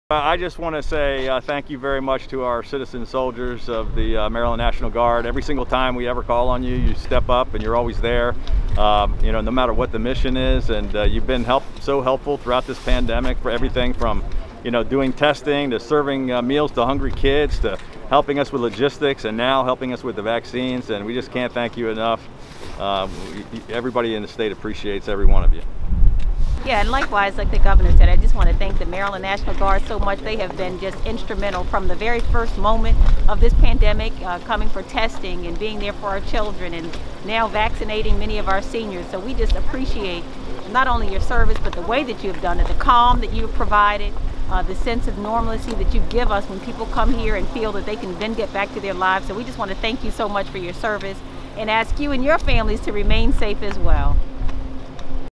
Gov. Hogan and PG County Executive Give thanks to MDNG
Governor Larry Hogan and Prince George's County Executive Angela D. Alsobrooks say thank you to the MDNG for their services during the opening of the Mass Vaccination site at Six Flags America in Bowie, Md. Feb. 5, 2021. The Maryland National Guard has been activated to support Maryland's COVID-19 vaccination initiative inoculating the public. Mobile Vaccination Support Teams provide direct support to county health departments, assisting with the acceleration of vaccination efforts.